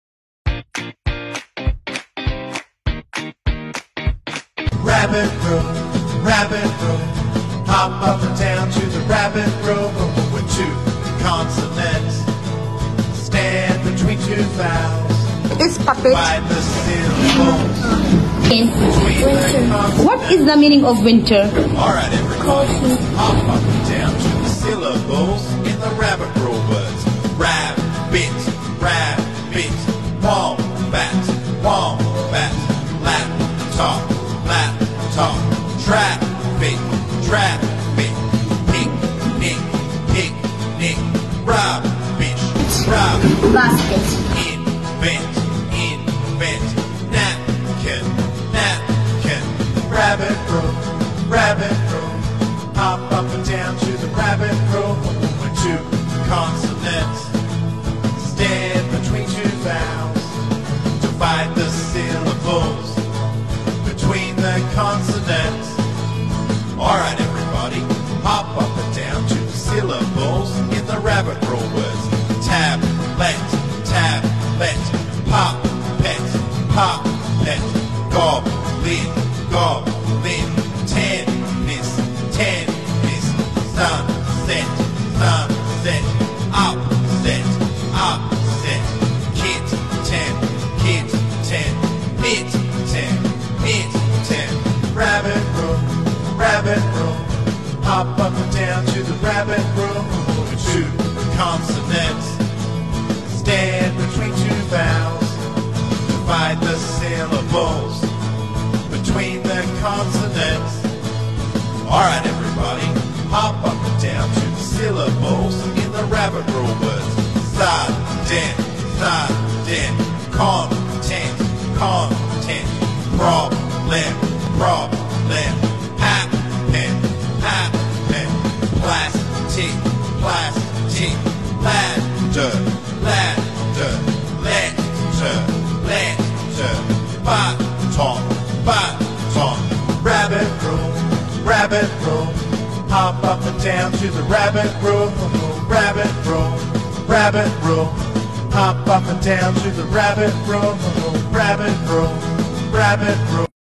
Reema_International_School Class 3M
"Mastering the VCCV Pattern: Grade 3 students sharpen their spelling skills by breaking down words into vowel-consonant-consonant-vowel chunks!"
Class 3M